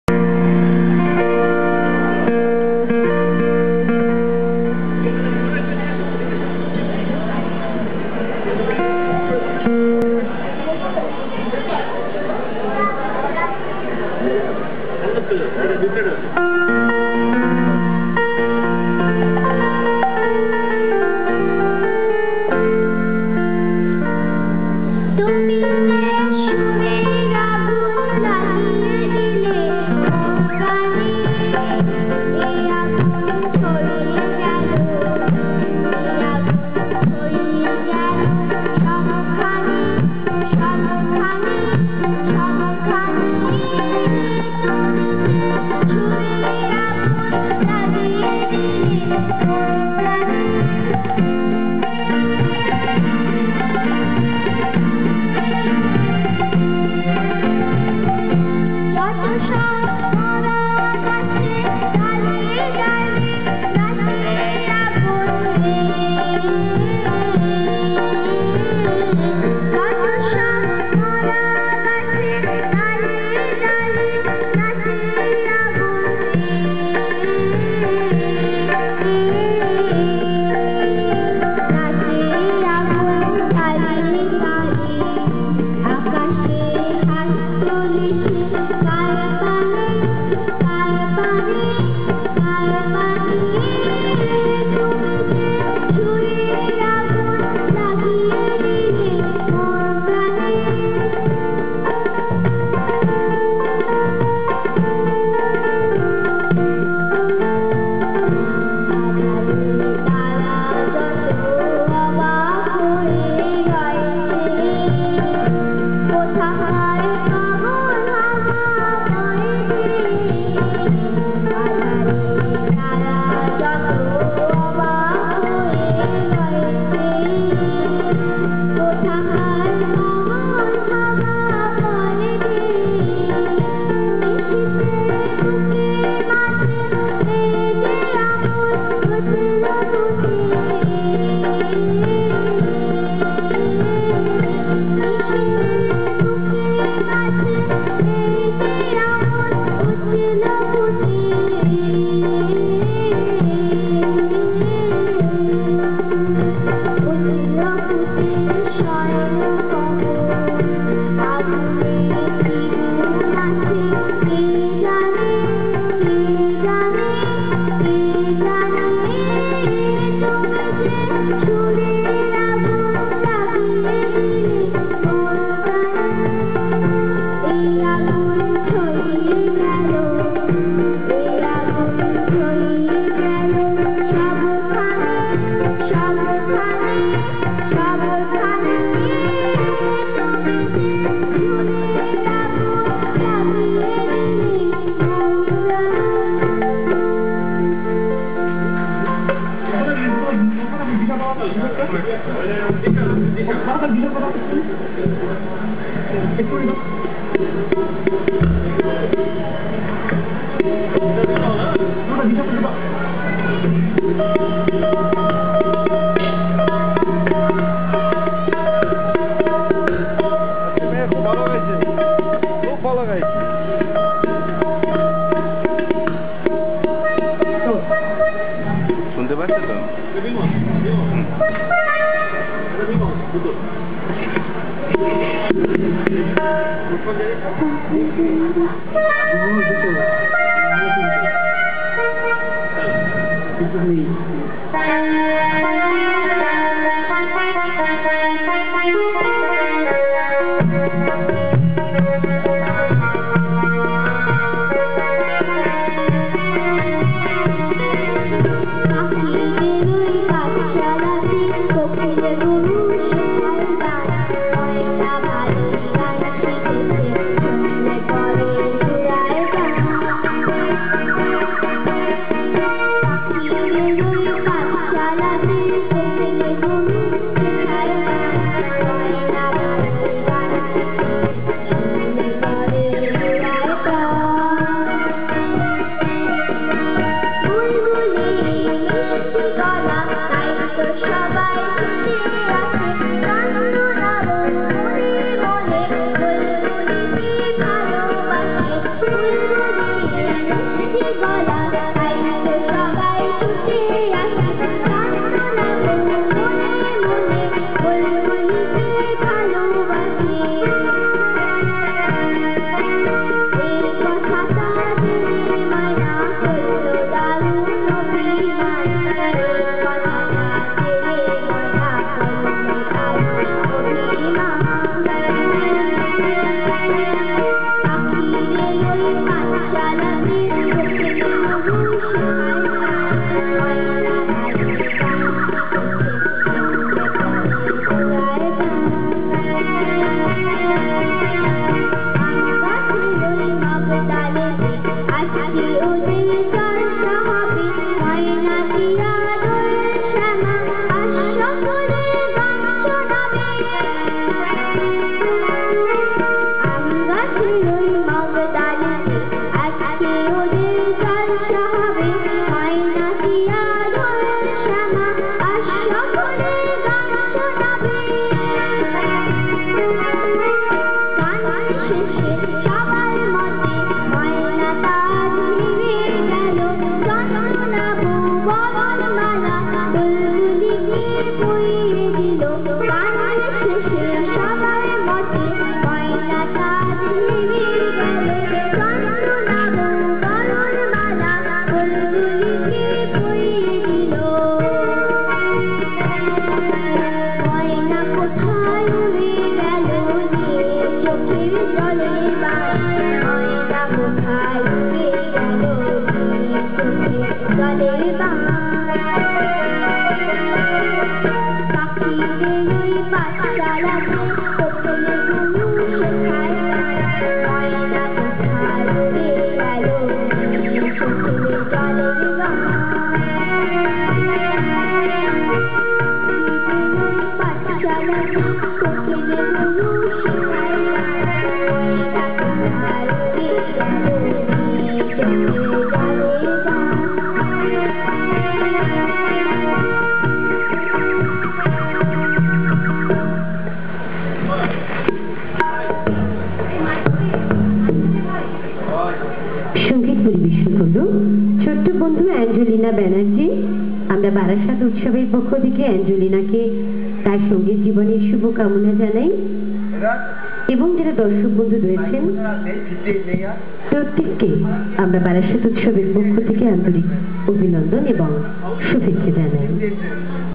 Barasat utsav 2010 Angelina's song.mpg
At Barasat utsav 2010 on 27th December 2010,the day her grandfather had a scerebral stroke,she sang excellently along with Bappa and his great troupe.
Thanks a lot Bappa for your music.